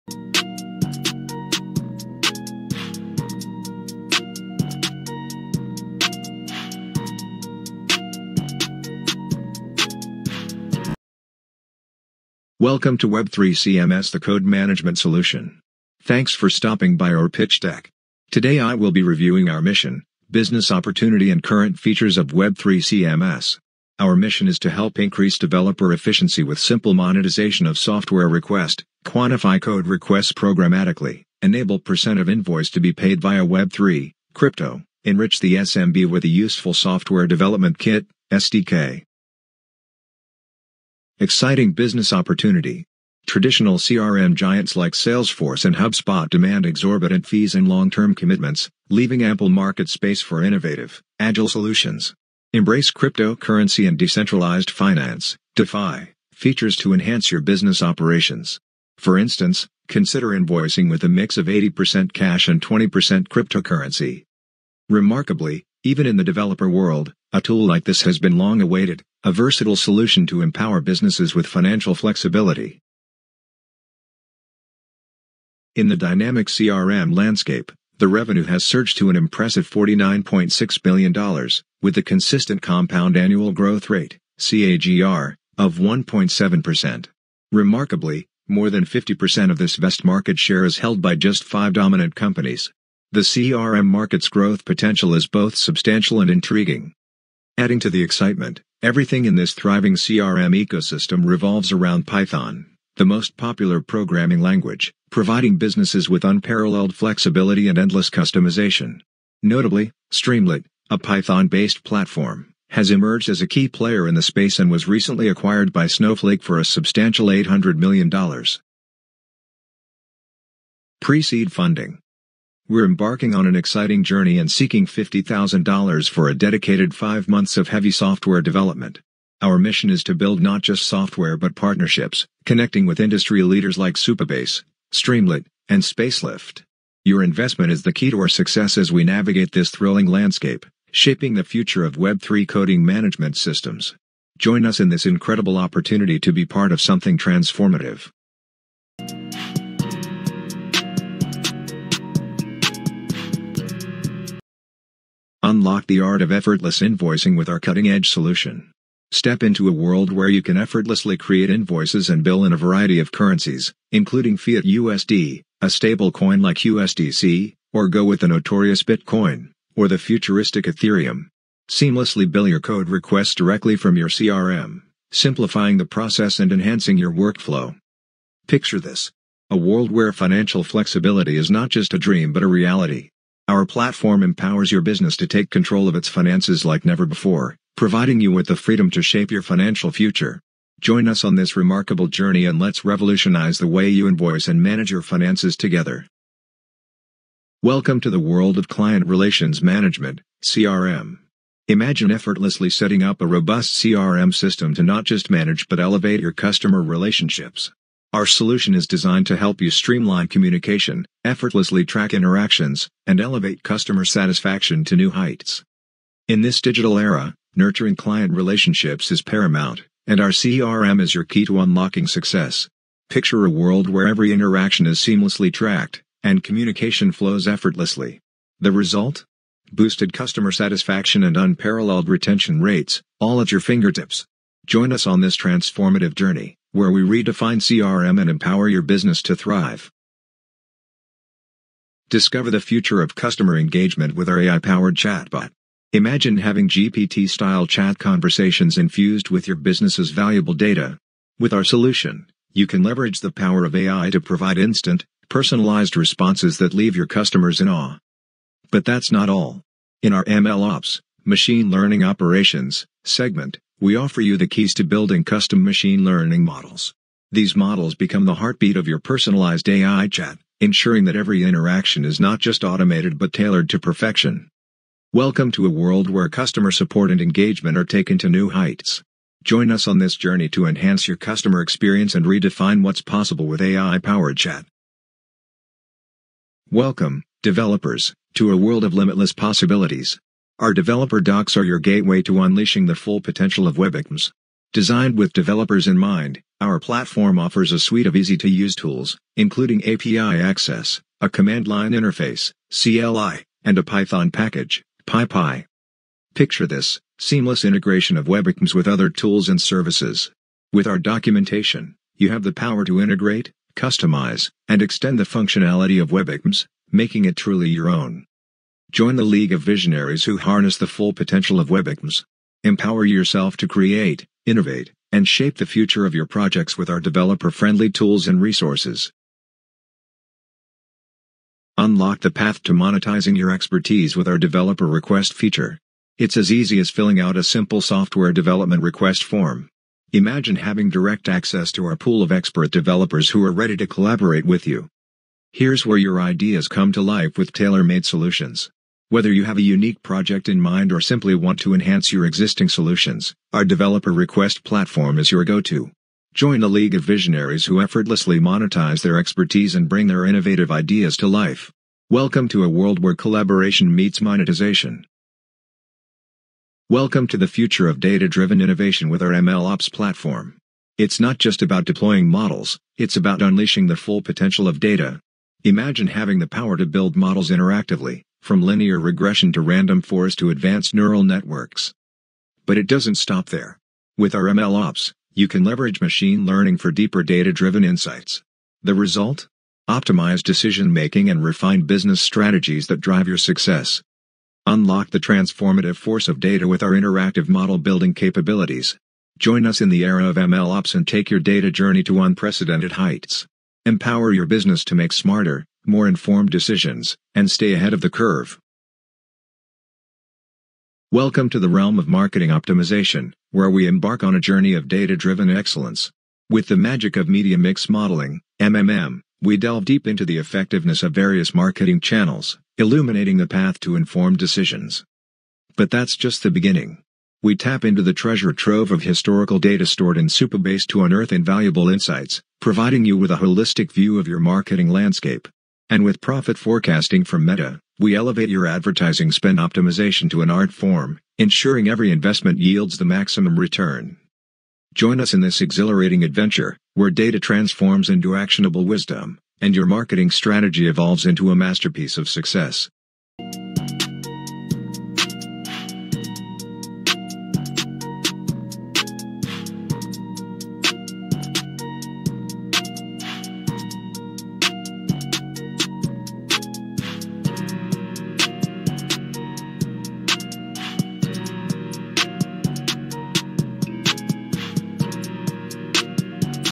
web3CMS (Code Management Solution) Pitch Deck 🌱
🌱 Dive into the future of code management with web3CMS - your all-in-one solution! Explore the web3CMS Pitch Deck and unleash the power of innovative features that include Invoicing, CRM, AI Chat, Dev Docs, Dev Request, Machine Learning Ops, and Marketing Optimization with Media Mixed Models.

Integrations:
Supabase 🌐
Stripe 💳
Streamlit 📈
Prophet of Meta 🔮
HuggingFace ML Models 🤖

0:00 web3CMS Intro
0:11 Mission
0:40 Market Opportunity
1:13 Market Size
1:59 PreSeed Funding
2:25 web3CMS Features
2:34 Invoicing
3:16 CRM
4:00 AI Chat
4:44 Dev Docs
5:30 Dev Requests
6:05 ML Ops
6:49 Media Mix Models
7:32 Visit web3CMS